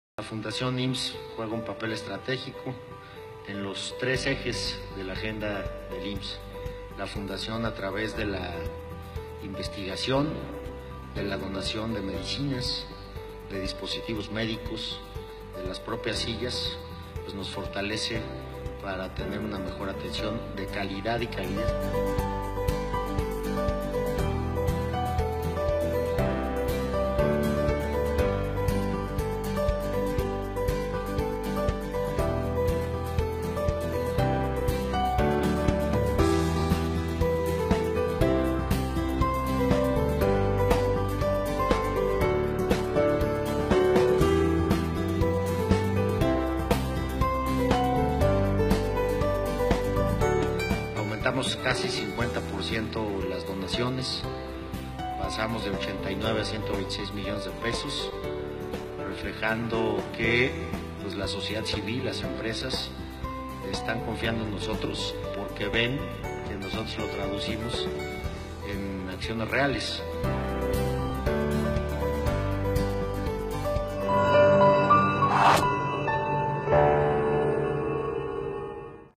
Aumentan 47 % Donativos a Fundación IMSS
En el presente año las prioridades de la Fundación son similares a las del IMSS: cuidar la salud financiera, apostar a la prevención, fortalecer la innovación e investigación, mejorar la calidad y calidez de los servicios médicos, fomentar la lactancia materna y acompañar las necesidades de IMSS-PROSPERA.